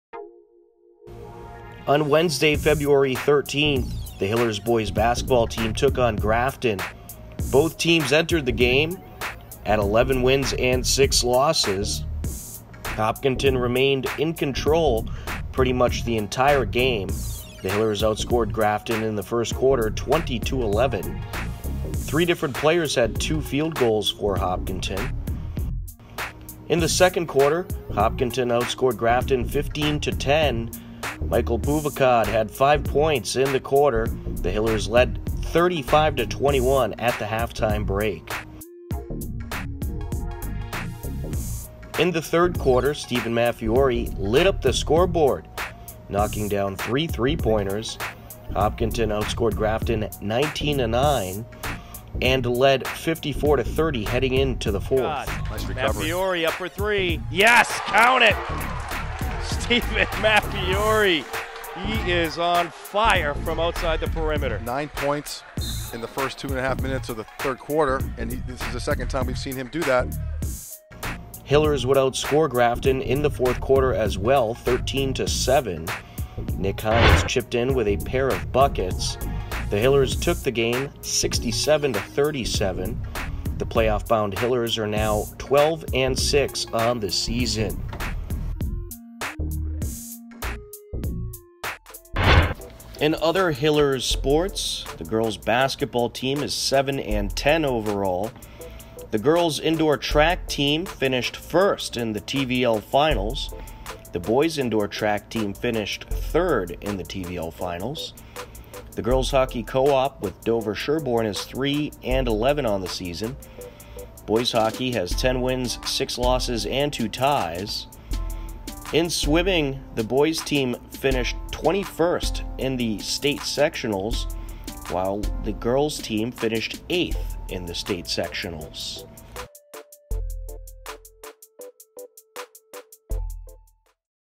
Hopkinton Hillers Sports Update 2-14-2019
Hopkinton Hillers Sports Update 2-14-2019 Featuring Boys Basketball Highlights VS Grafton